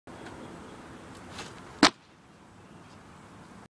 Redemption of first 5 foot power ollie
The last one was really sketchy and my hands touched the ground